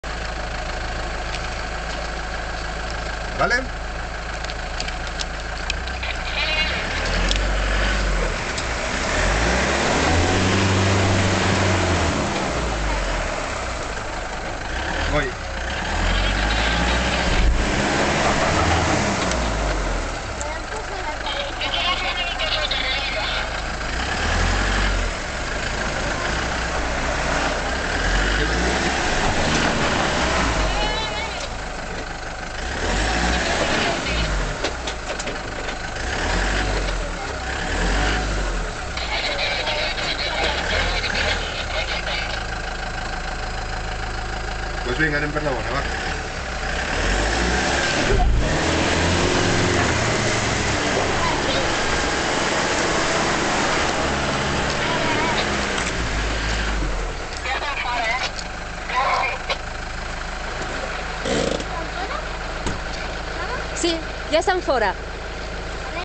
Atasco Anibal
El hielo no aguanta el peso, el fondo es pastoso, en el antes y el despues el camino esta congelado..